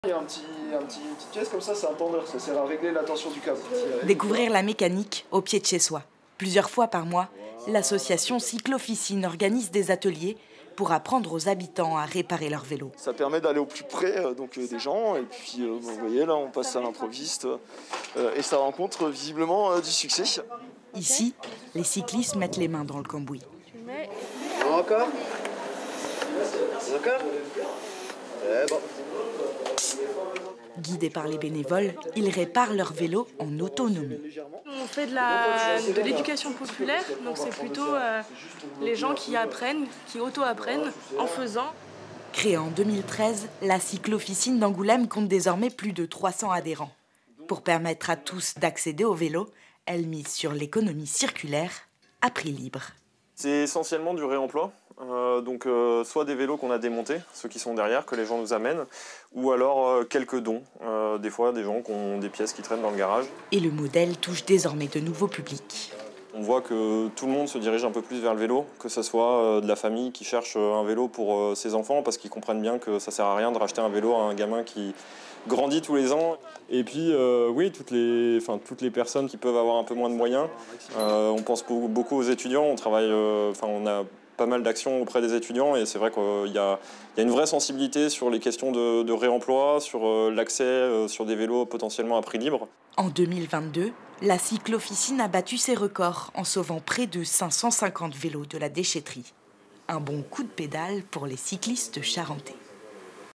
Ateliers : apprendre à réparer son vélo à domicile
Sauver des vélos, plutôt que de les jeter. C'est le principe de l'association Cyclofficine qui propose des ateliers d'auto-réparation des vélos. Plusieurs fois par mois, elle se délocalise pour aller à la rencontre des habitants. Reportge dans le quartier de la grande garenne à Angoulême avec Lisa Macinairas et Pascal Montagne
Intervenants :
- Dav Hojka, administrateur de la Cyclofficine d'Angoulême
- Élodie Mourier, salariée de la Cyclofficine d'Angoulême
- Léo Chabert, administrateur de la Cyclofficine d'Angoulême